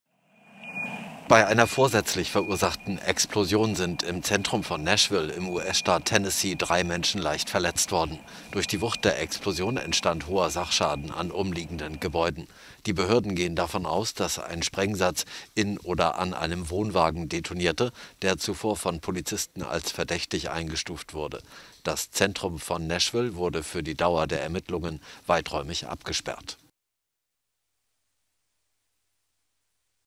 Nashville: Hoher Sachschaden und Verletzte nach mysteriöser Explosion
Nach ersten Erkenntnissen der Polizei war eine noch unbekannte Art von Sprengsatz in einem Wohnwagen im Zentrum der 700.000-Einwohner-Stadt Nashville in die Luft geflogen. Die Polizei war zuvor zum Explosionsort gerufen worden.